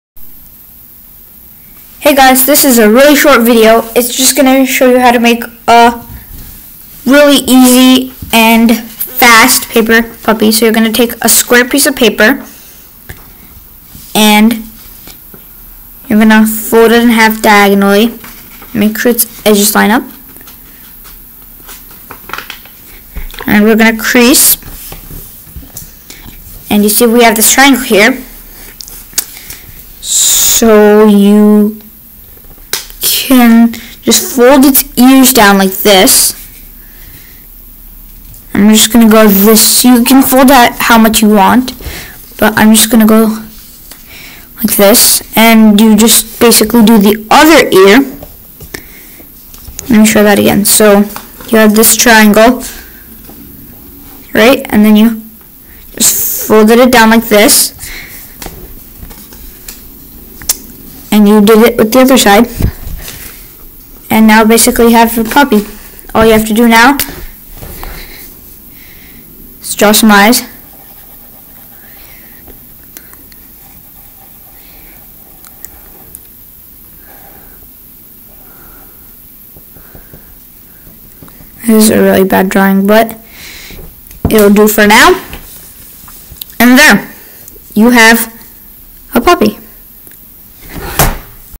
Short Origami Puppy Tutorial
This is a short video where I tell you how to fold a quick and easy paper puppy.